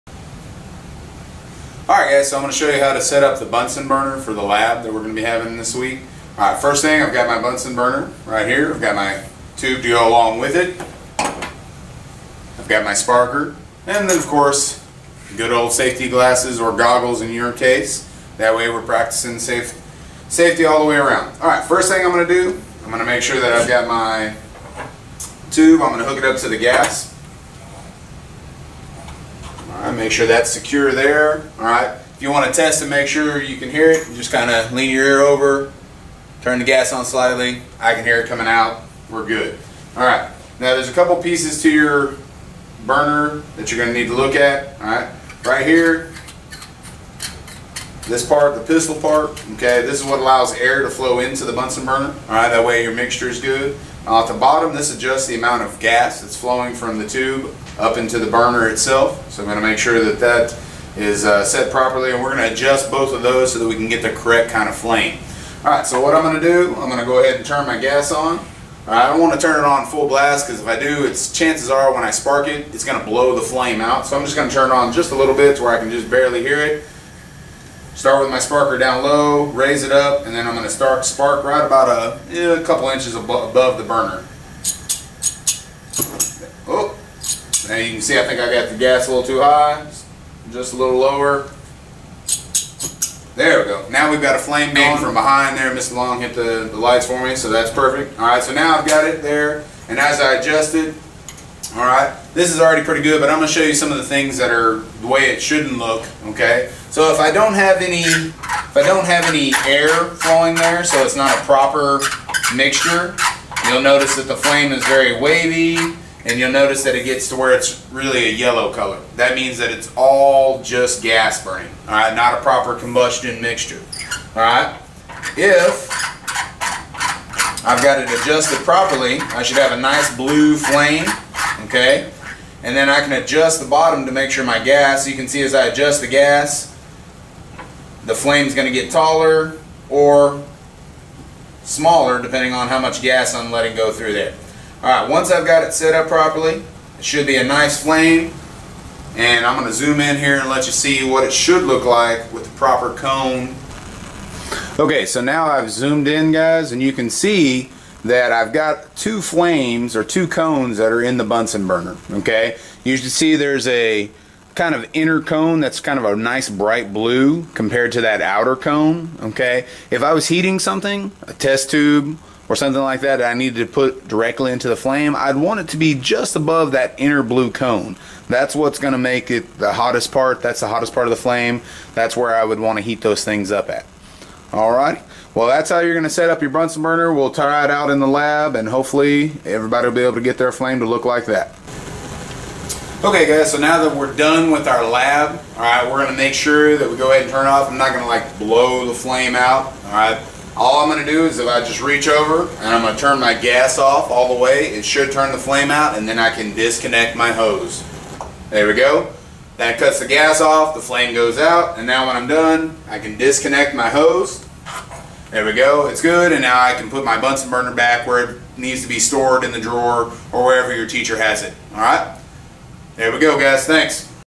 Bunsen Burner Lab Video
How to properly set up, light, and adjust a Bunsen Burner.